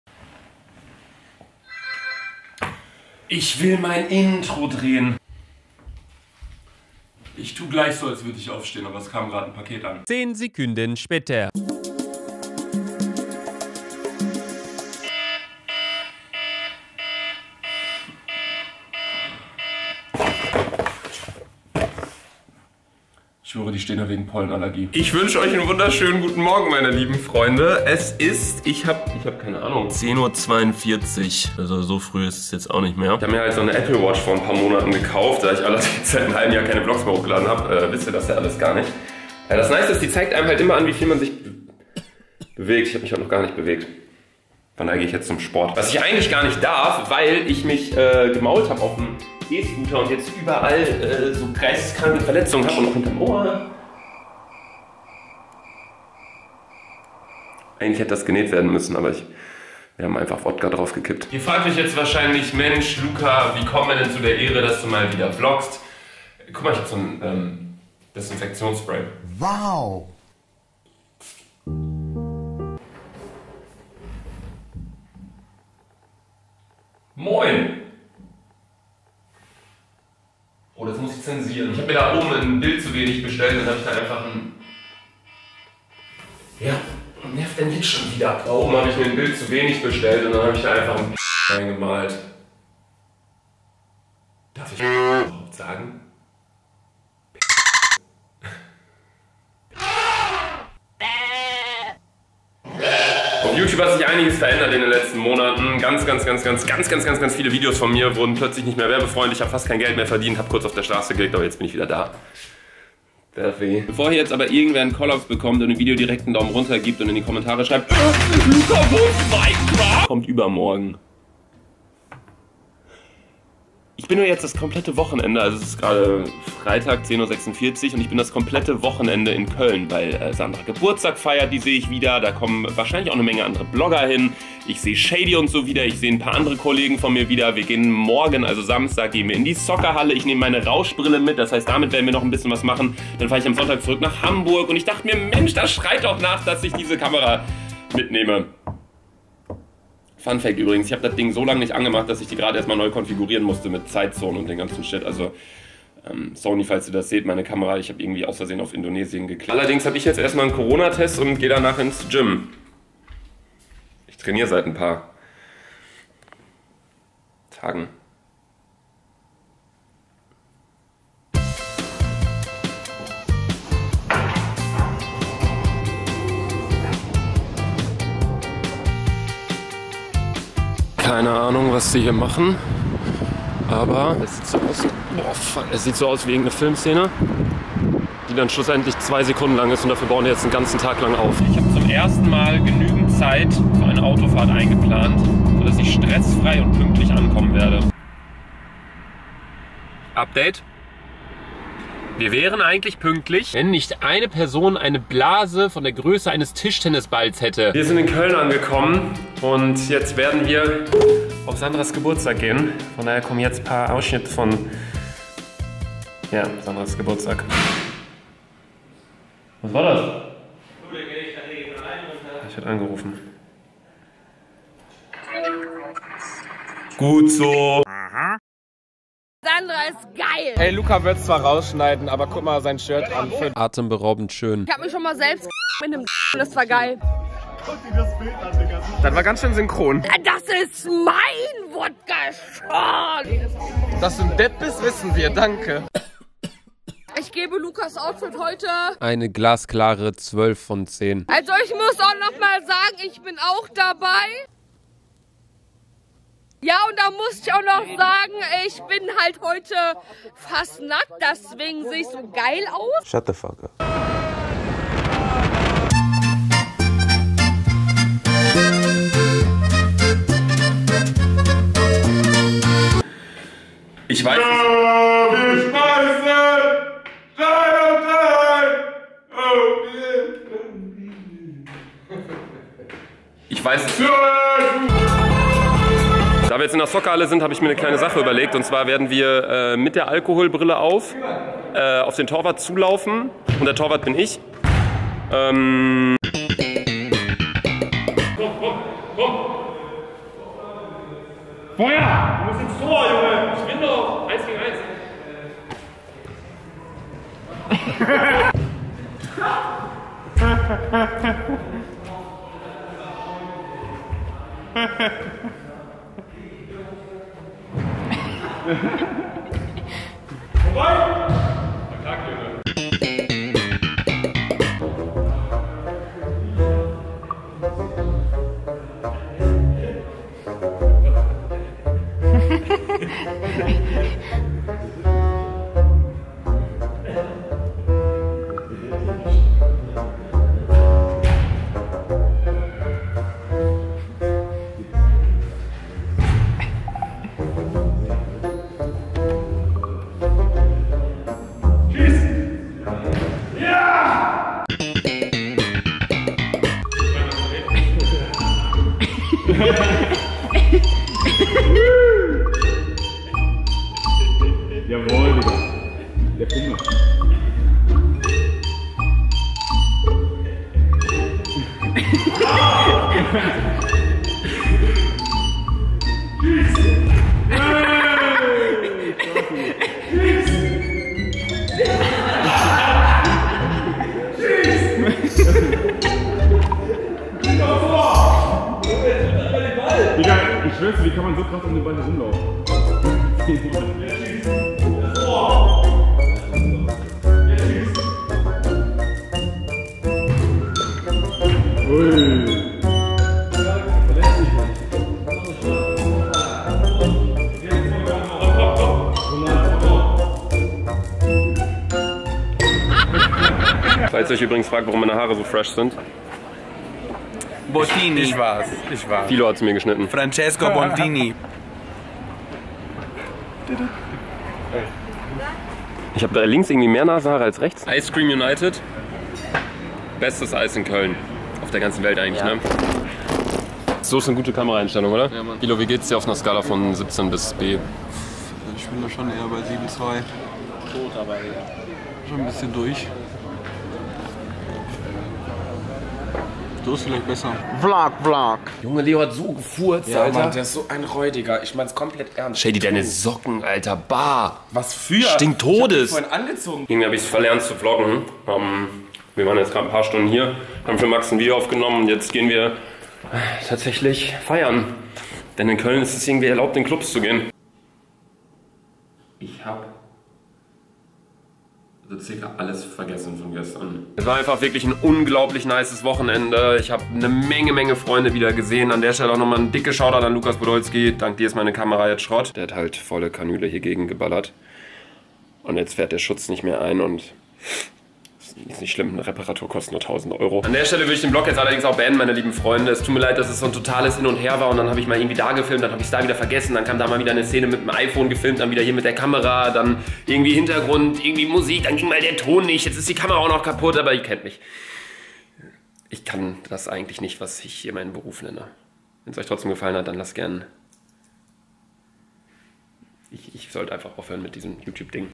DER DÜMMSTE VLOG ALLER ZEITEN
ich war mit freunden in köln und habe sandra auf ihrem geburtstag besucht, war mit lukas podolski und meiner alkohol brille in der soccerhalle und habe vieles dummes zeug gemacht lol